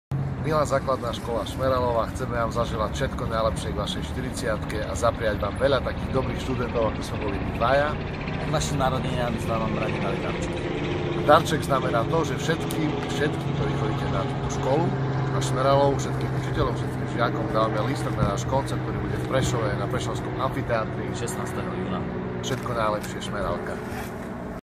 Ivan Tásler & Miro Tásler - 40. výročie ZŠ Prešov, Šmeralova
K 40. výročiu založenia školy darovali Miro a Ivan Táslerovci celej škole ZŠ Šmeralova v Prešove vstupenky na koncert v prešovskom amfiteátri v rámci IMT SMILE Amfiteáter Tour 2018.